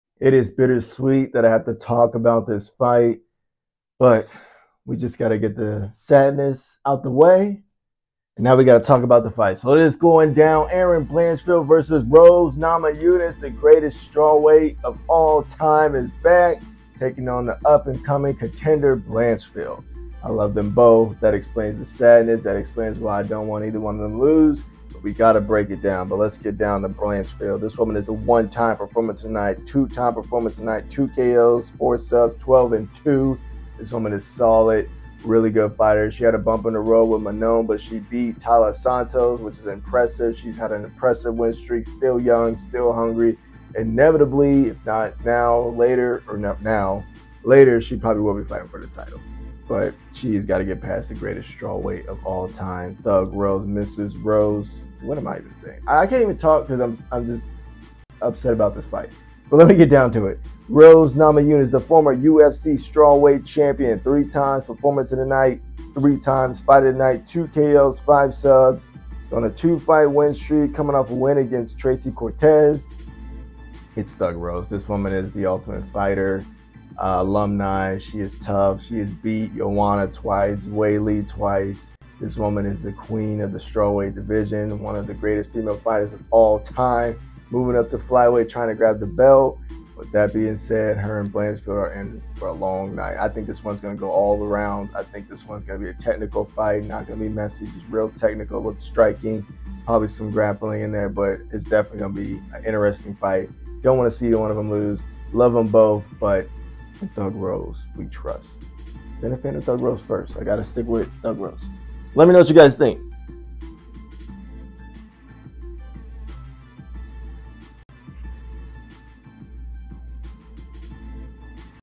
Rose Namajunas vs Erin Blanchfield
Its bitter sweet that these two have to fight 🫥😢

Love em both but im riding with thug rose since ive been a fan since the ultimate fighter !